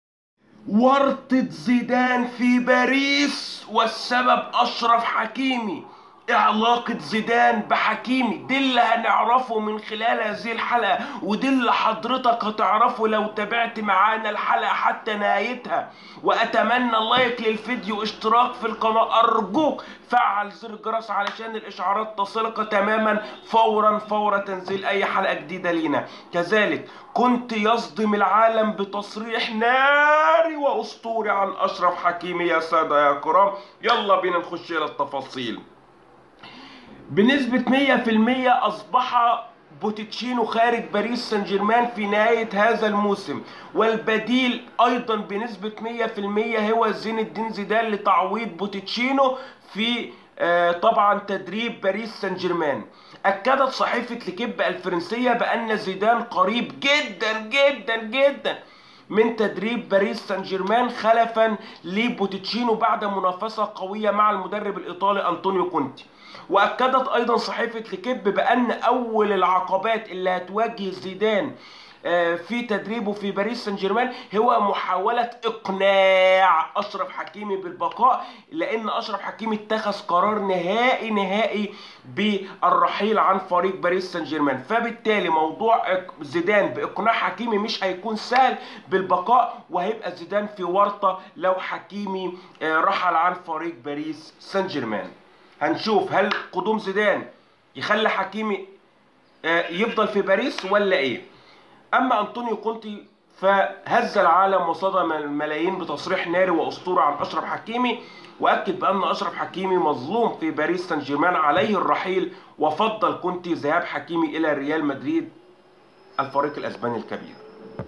ورطة زيدان فى باريس والسبب أشرف حكيمى وكونتى يصدم العالم بتصريح نارى وأسطورى عن حكيمى 🔥🔥لن تصدق أبدا
أتمنى تدعمنى وتشرفنى بالاشتراك فى قناتى وبالتوفيق والنجاح دايما ❤ ❤